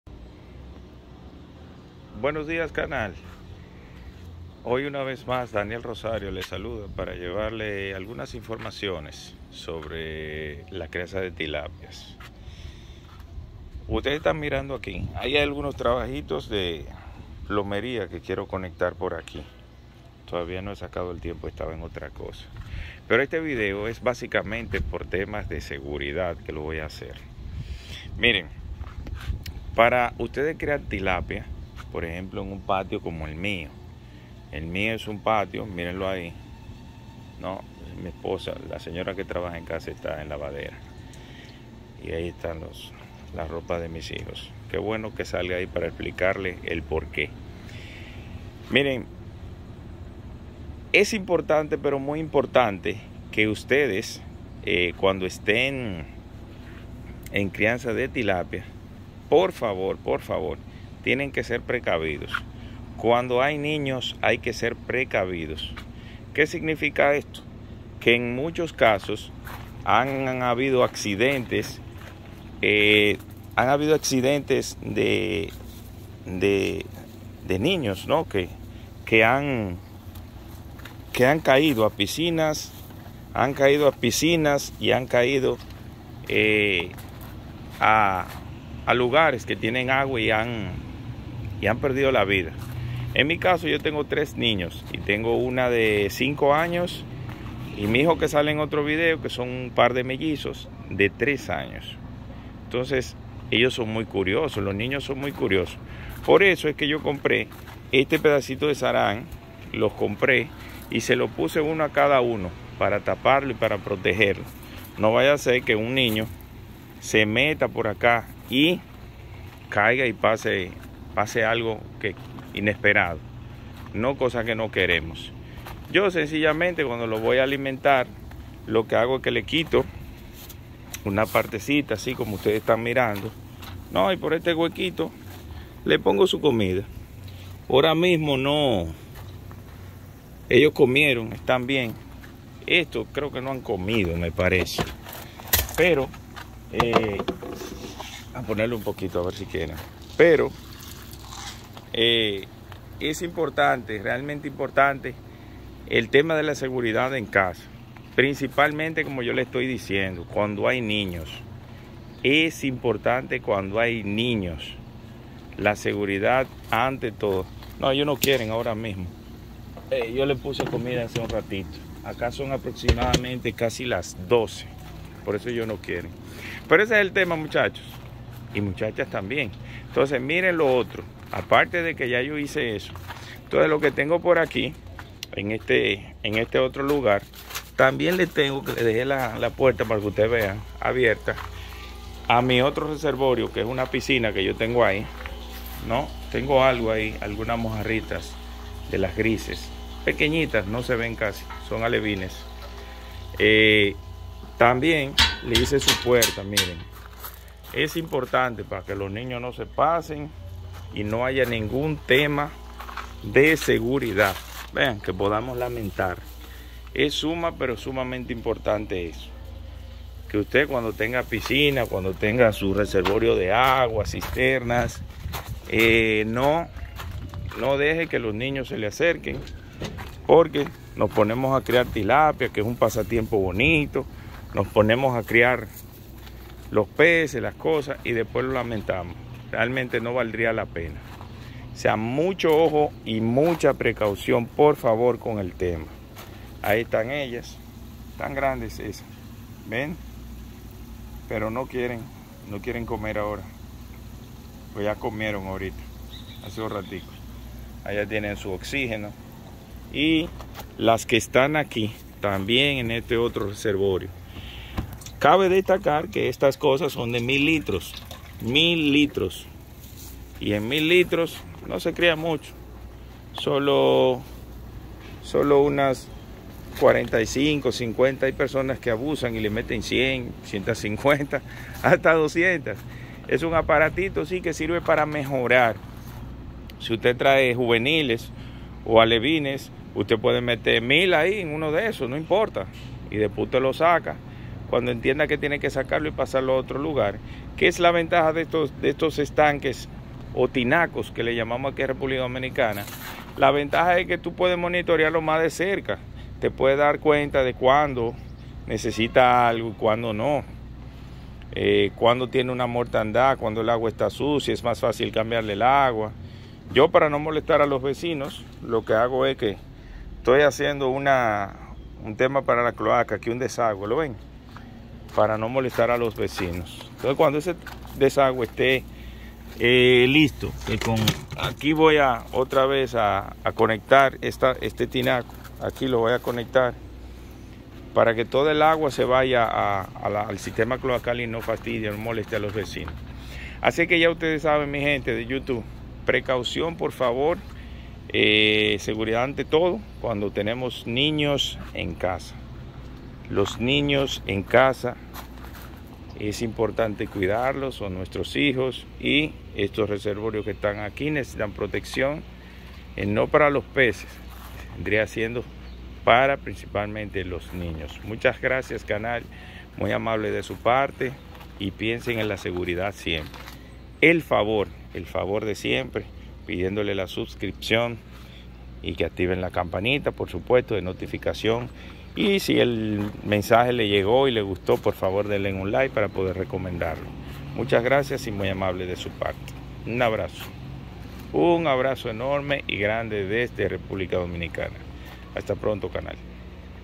Las piscinas o estanques y su peligro para los niños.
La seguridad de los niños en el hogar es una de las preocupaciones más importantes de todo padre o tutor. En particular, cuando se trata de piscinas y reservorios de agua que se utilizan en casa, es fundamental tomar medidas para garantizar la seguridad y protección de los más pequeños.

Para ello, es necesario implementar medidas de prevención y seguridad que reduzcan el riesgo de ahogamiento y otros accidentes. Algunas opciones incluyen cercar la piscina o el reservorio con una valla segura, colocar alarmas en las puertas de acceso al área de la piscina o al reservorio, y enseñar a los niños sobre los peligros del agua y la importancia de pedir ayuda en caso de emergencia.

También es importante asegurarse de que los niños estén supervisados en todo momento mientras se encuentran en el área de la piscina o el reservorio. Los responsables deben estar atentos y mantener una constante vigilancia para evitar cualquier situación de riesgo.

En resumen, la seguridad de los niños es una responsabilidad compartida por todos los miembros de la familia. La implementación de medidas de prevención y la supervisión constante son fundamentales para garantizar la protección de los más pequeños en el hogar, especialmente en el entorno acuático de las piscinas y reservorios con algún fin.